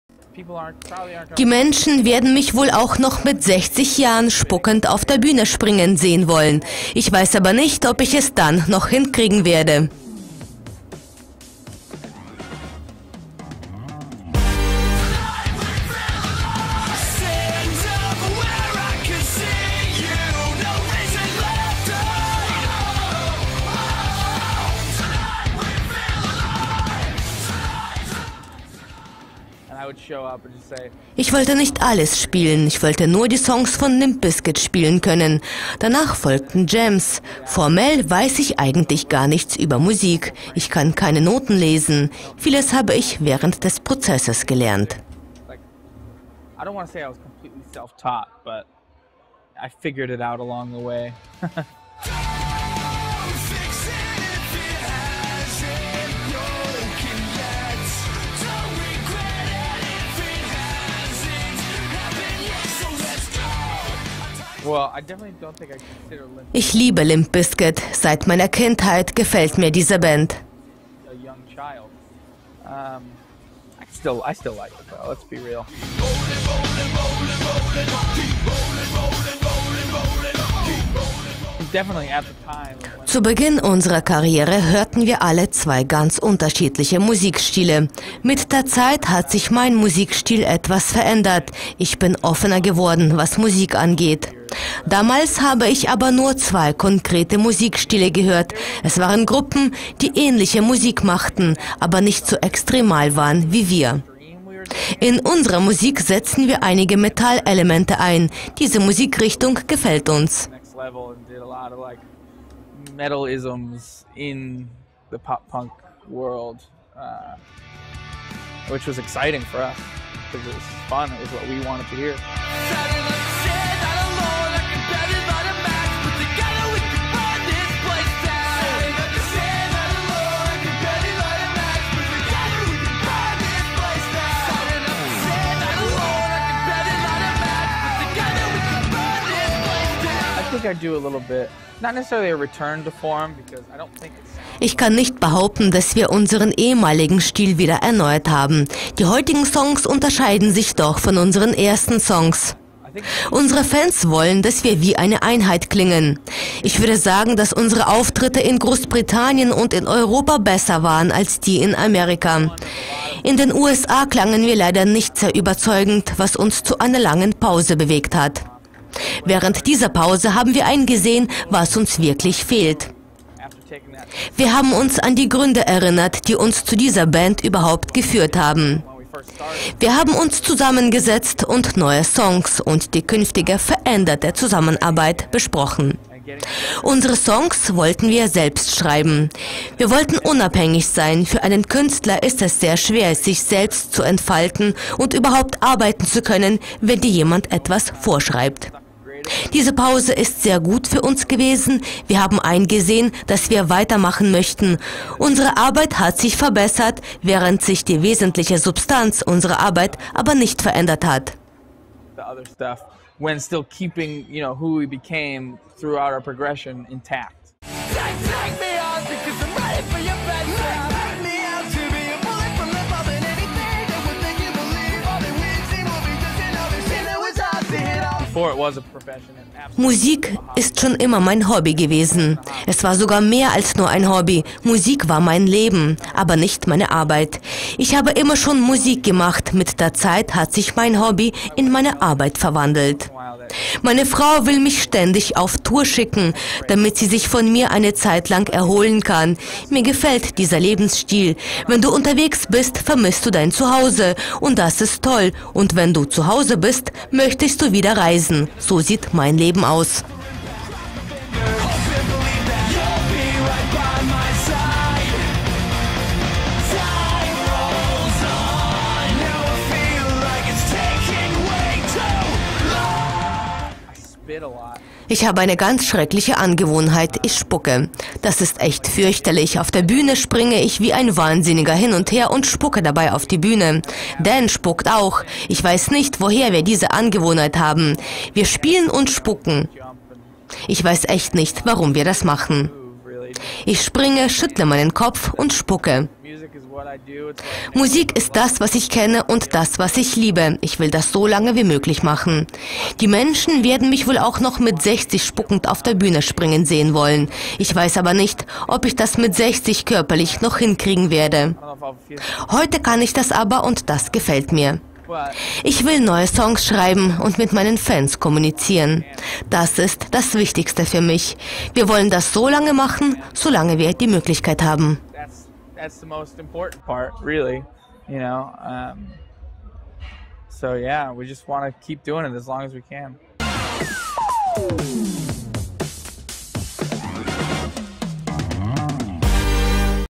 Four Year Strong. Impromptu
Alan Day, der Frontman der Musigruppe Four Year Strong  spricht über das Spucken auf der Bühne in einem Alter von 60 Jahren und wie die Band einen Neuanfang geschafft hat.